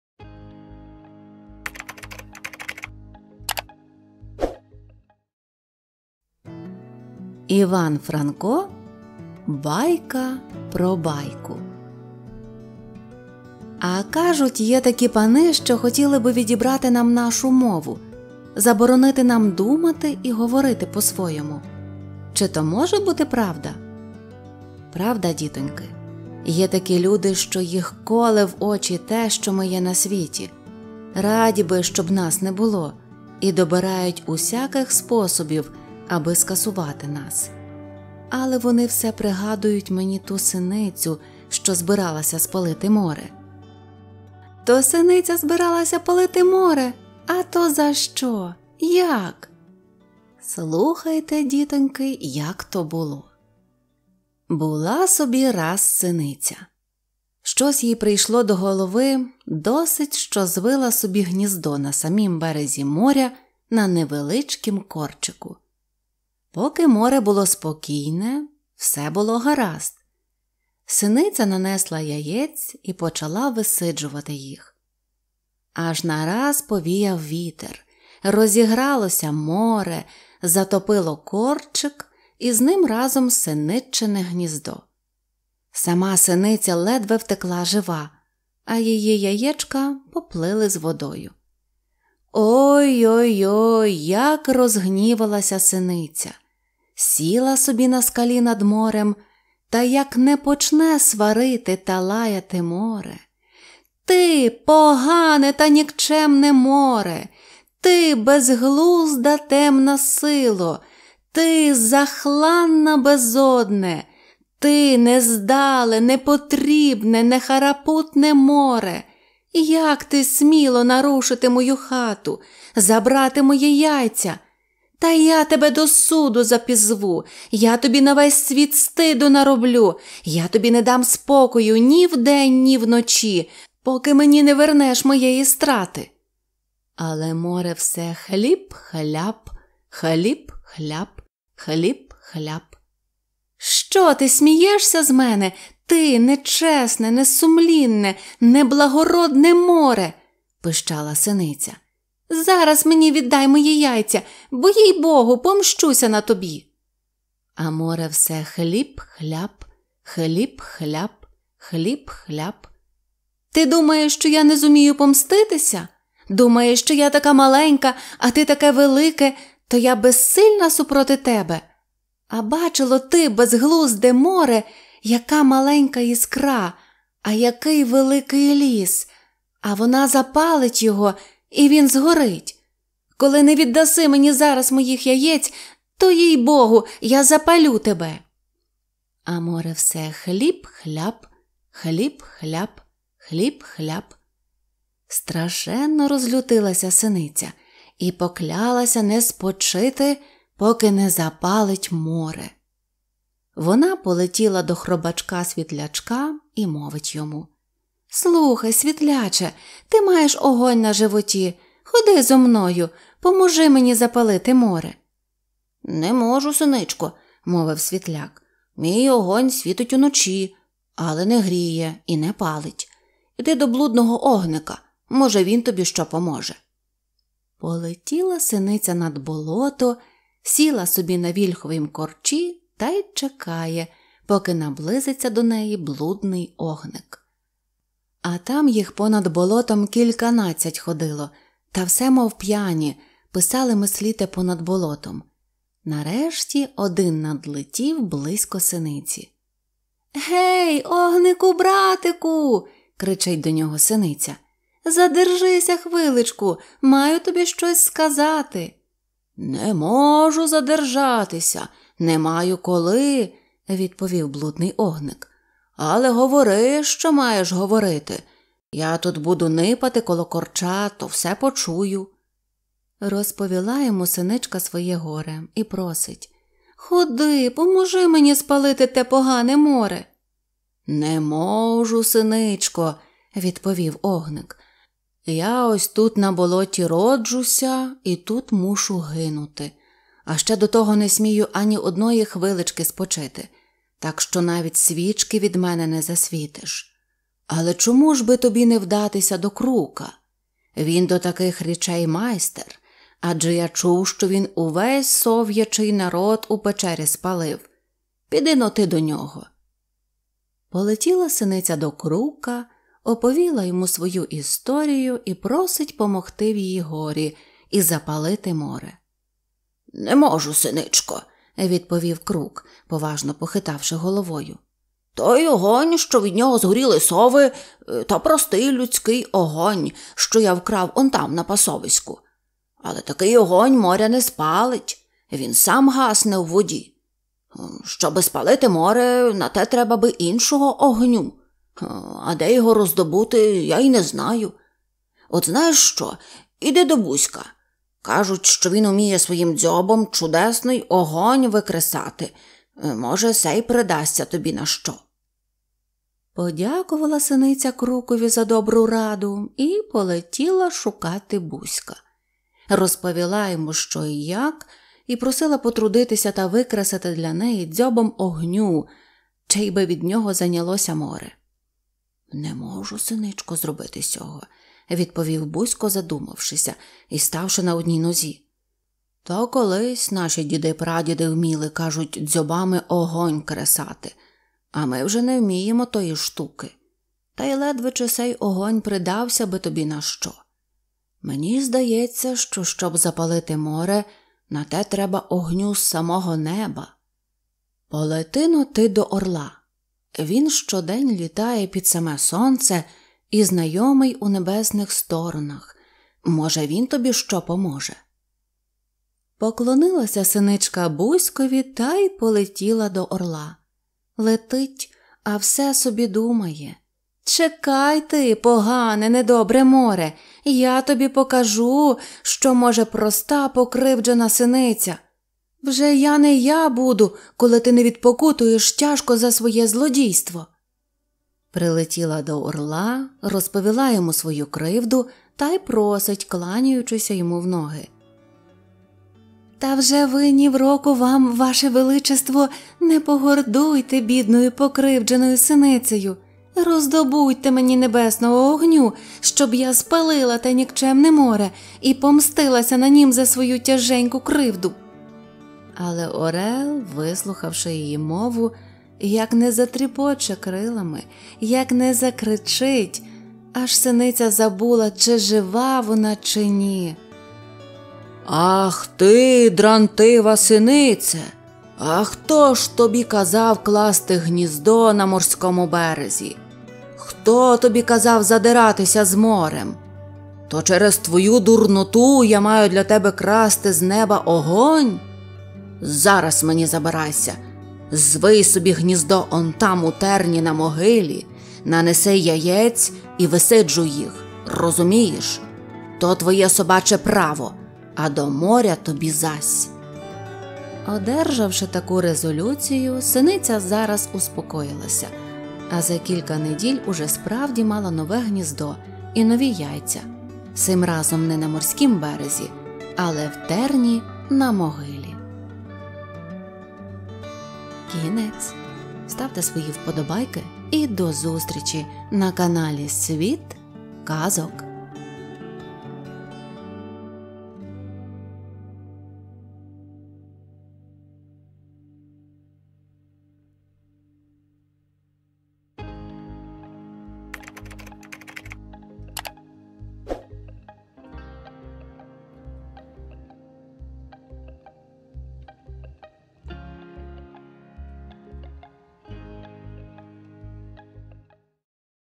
ЯК СИНИЦЯ ХОТІЛА СПАЛИТИ МОРЕ (Іван Франко) БАЙКА ПРО БАЙКУ - аудіоказка українською мовою
— Правда, дітоньки. Є такі люди, що їх коле в очі те, що ми є на світі, раді би, щоб нас не було, і добирають усяких способів, аби скасувати нас. Але вони все пригадують мені ту синицю, що збиралася спалити море.
— О, о, о! То синиця збиралася палити море? А то за що? Як?
— Слухайте, дітоньки як то було!

(Іван Франко «Як Синиця хотіла море спалити»)